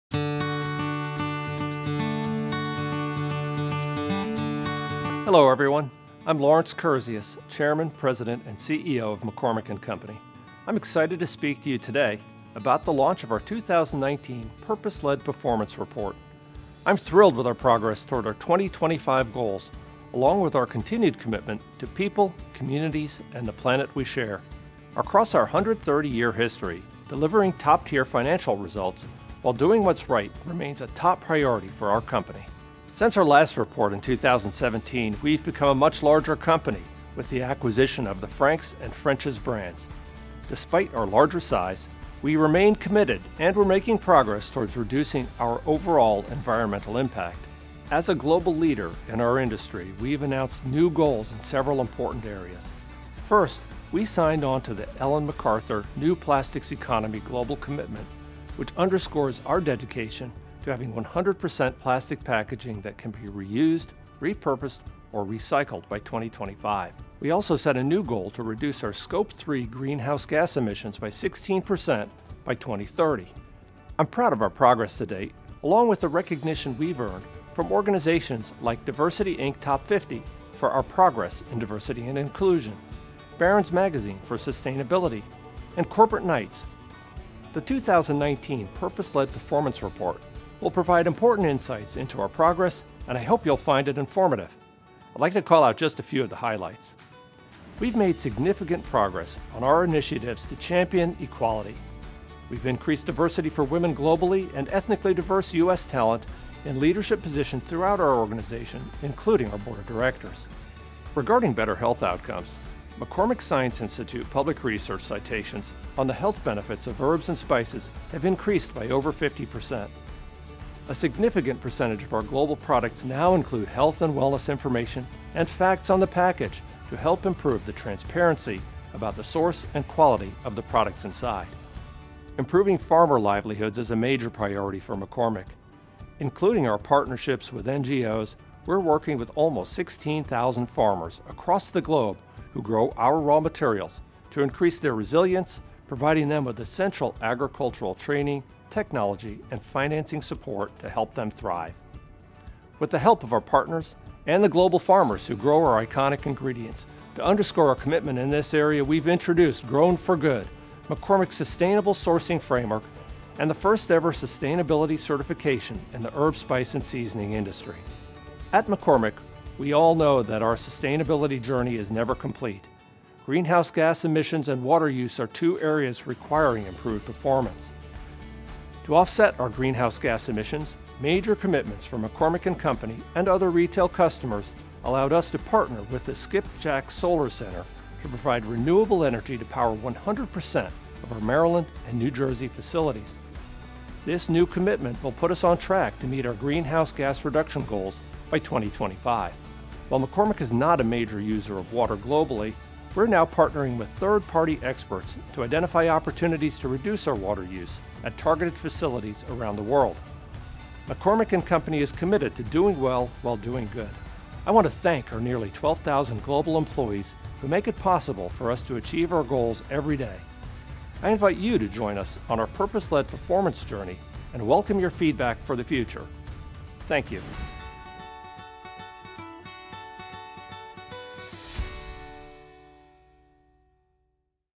Introduction to Purpose-led Performance 2019
We're proud to launch our 2018 Performance-led Performance Report, reflecting our work to improve the health and well-being of all people; to build vibrant communities where we live, work, and source; and to make a positive impact on the planet.